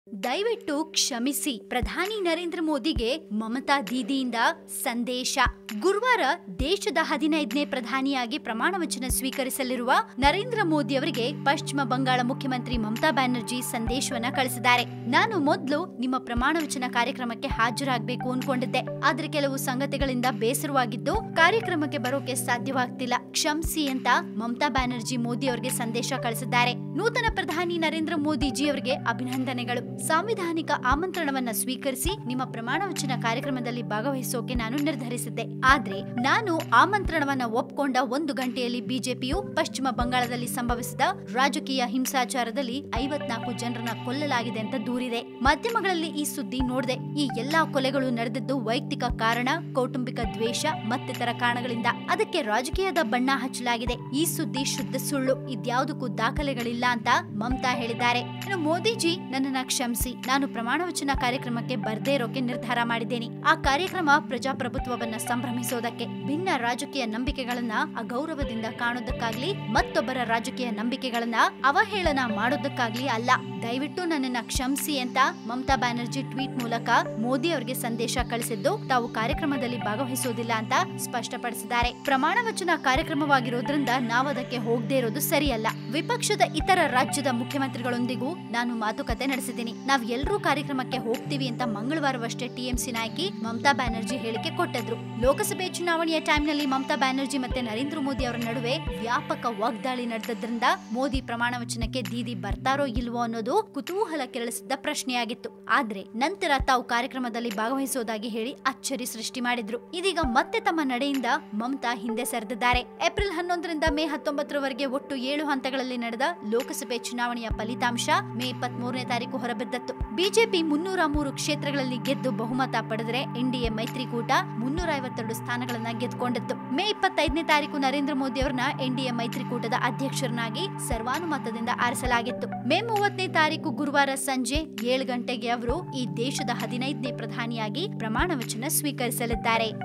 ಪ್ರಧಾನಿ ನರೇಂದ್ರ ಮೋದಿಗೆ ಸಂದೇಶ ಕಳುಹಿಸಿದ ಮಮತಾ ಬ್ಯಾನರ್ಜಿ
West Bengal CM Mamata Banerjee: It was my plan to attend oath-taking ceremony, however in past one hour, I am seeing media reports that the BJP is claiming 54 people have been killed in political violence in Bengal. This is untrue. I am compelled not to attend the ceremony.
 ಗುರುವಾರ ದೇಶದ 15 ಪ್ರಧಾನಿಯಾಗಿ ಪ್ರಮಾಣವಚನ ಸ್ವೀಕರಿಸಲಿರುವ ನರೇಂದ್ರ ಮೋದಿ ಅವರಿಗೆ ಪಶ್ಚಿಮ ಬಂಗಾಳ ಮುಖ್ಯಮಂತ್ರಿ ಮಮತಾ ಬ್ಯಾನರ್ಜಿ ಸಂದೇಶ ಕಳಿಸಿದ್ದಾರೆ. "ನಾನು ಮೊದಲು ನಿಮ್ಮ ಪ್ರಮಾಣವಚನ ಕಾರ್ಯಕ್ರಮಕ್ಕೆ ಹಾಜರಾಗಬೇಕು ಎಂದುಕೊಂಡಿದ್ದೆ. ಆದರೆ ಕೆಲವು ಸಂಗತಿಗಳಿಂದ ಬೇಸರವಾಗಿದ್ದು, ಕಾರ್ಯಕ್ರಮಕ್ಕೆ ಆಗಮಿಸಲು ಸಾಧ್ಯವಾಗುತ್ತಿಲ್ಲ. ಕ್ಷಮಿಸಿ" ಎಂದು ಮಮತಾ ಬ್ಯಾನರ್ಜಿ ಮೋದಿಯವರಿಗೆ ಸಂದೇಶ ಕಳಿಸಿದ್ದಾರೆ.